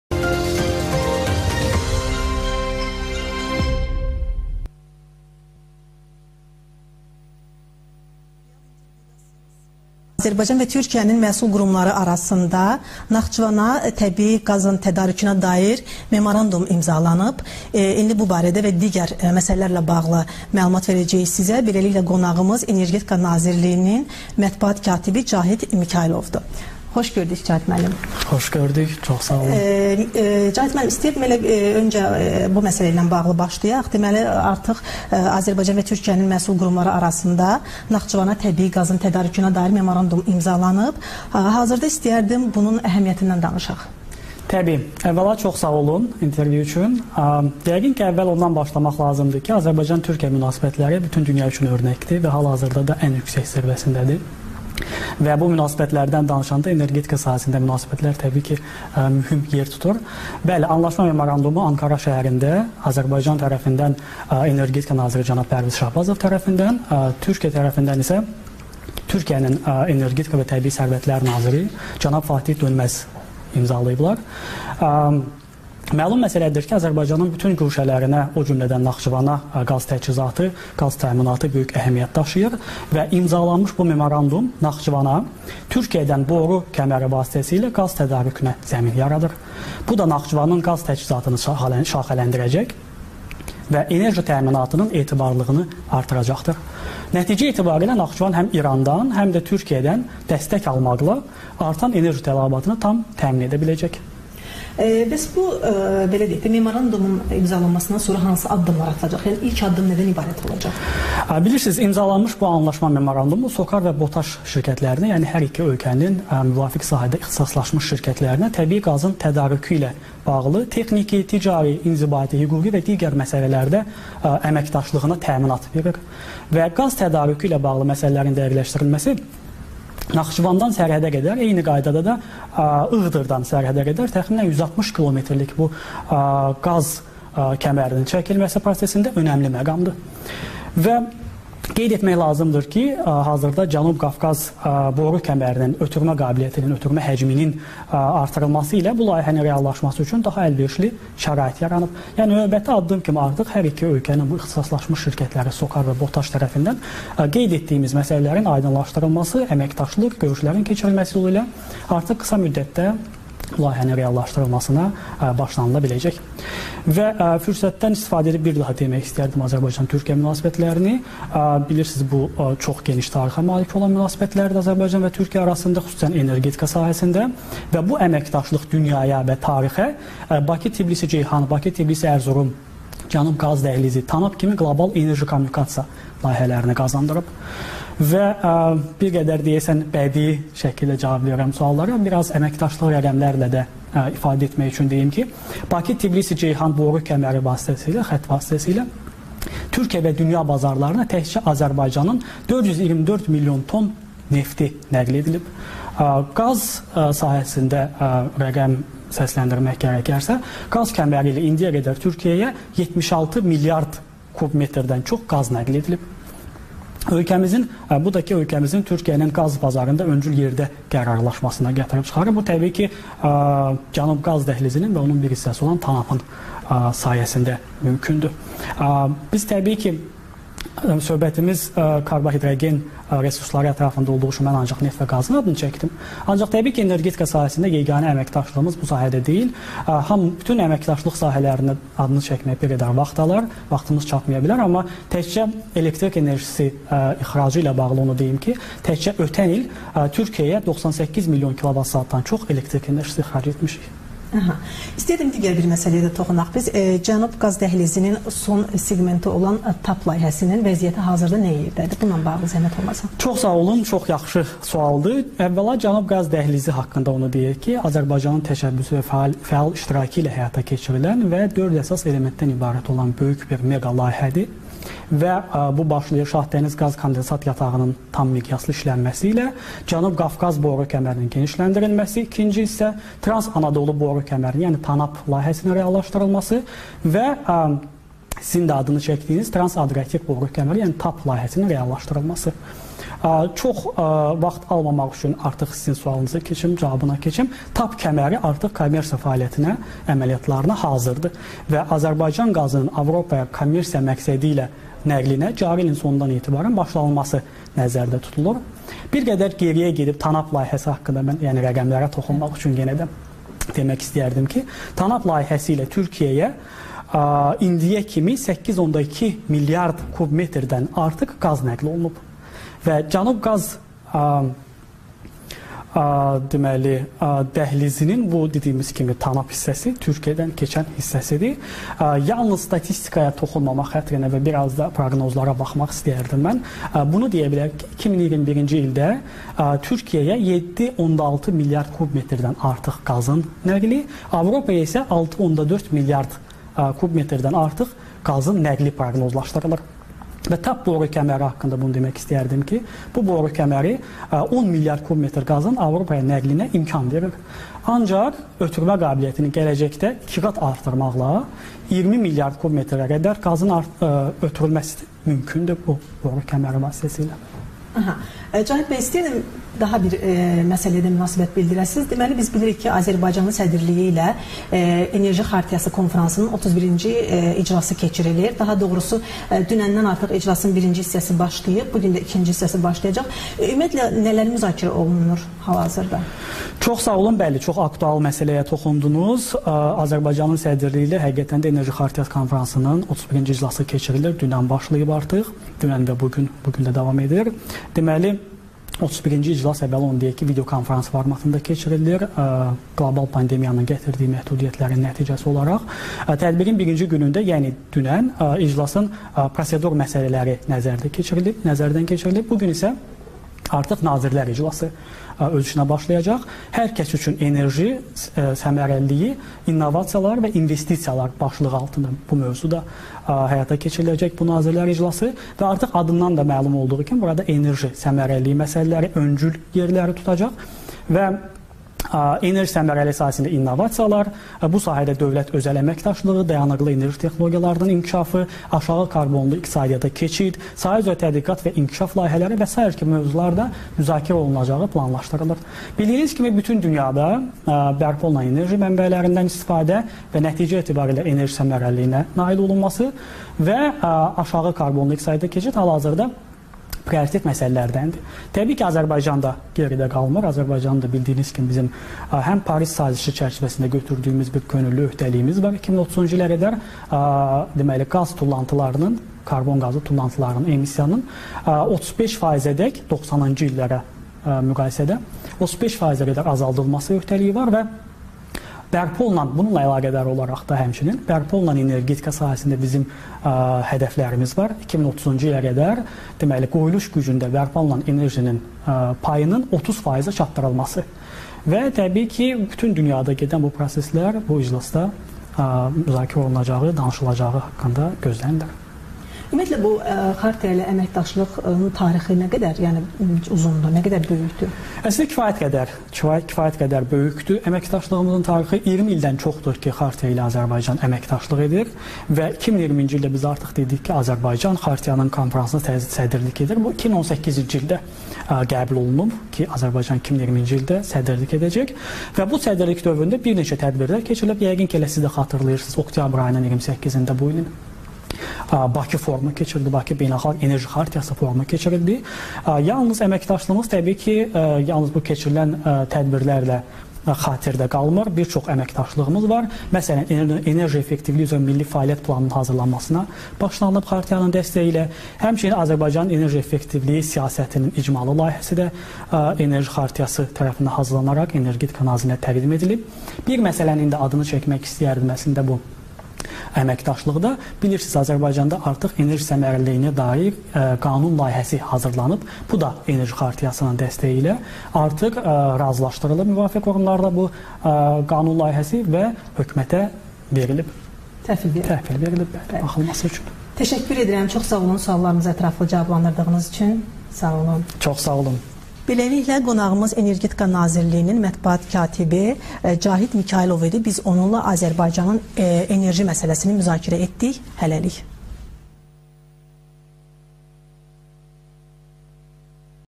Naxçıvana qaz təchizatı
Təbii qazın tədarükünə dair ilk addım atılıb